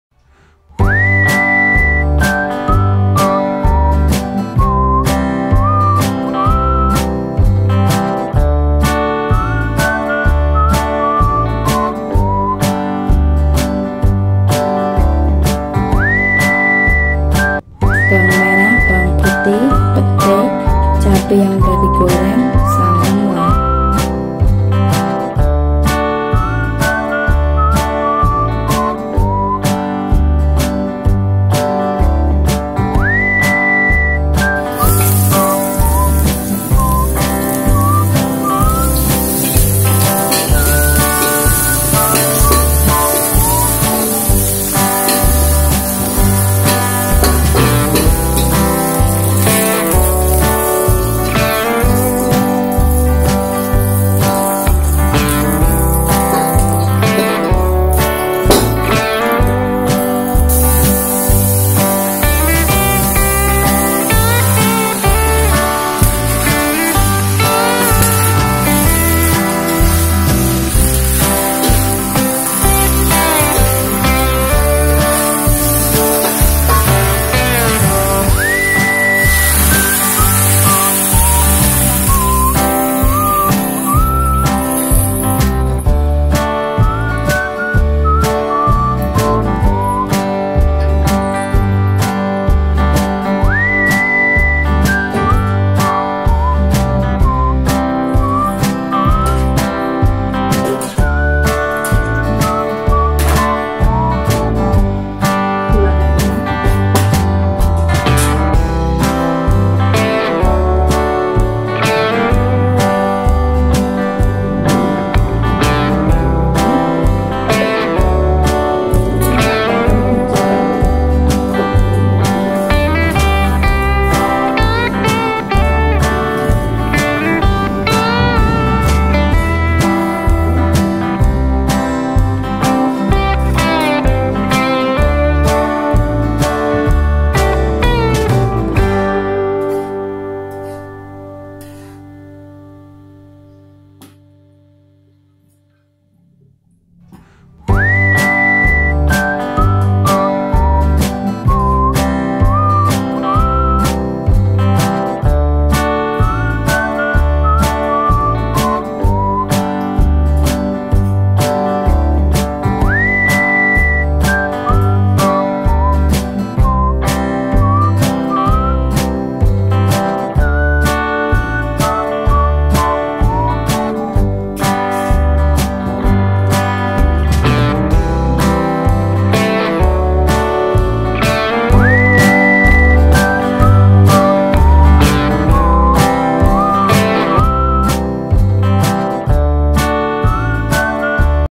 MEMASAK SAYUR LOMBOK WONOGIRI
Yang orang wonogiri pasti kenal Sayur/Jangan Tempe Lamtoro ini. Kemarin dapat oleh-oleh dari wonogiri ada : Tempe Kedelai, Tahu, Tempe Lamtoro dan petai terus langsung aku masak deh. Tempe Lamtoro ini harganya lebih mahal ya bila dibandingkan dengan tempe kedelai. Oya aku punya tips biar awet menyimpan tempe lamtoro, biasanya aq simpan di freezer jadi tahan sampai lama.